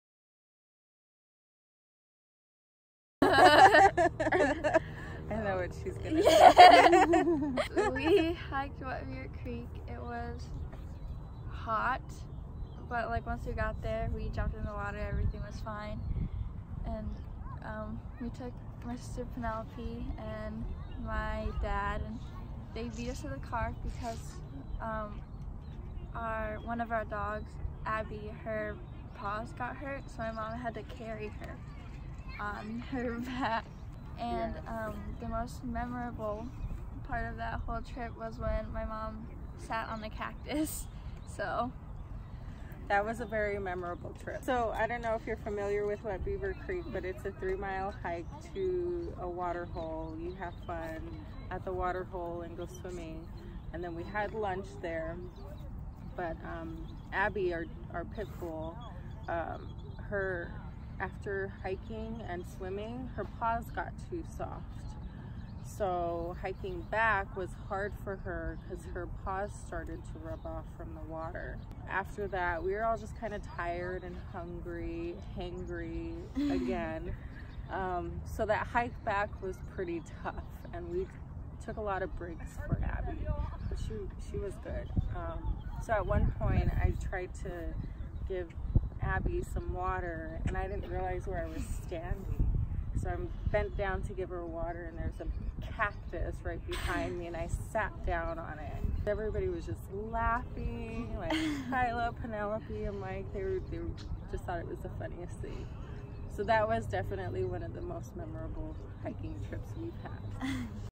Camelback Culture Community Spotlight: Hooper Family Memorable Hike
The ladies laugh and describe a memorable family hike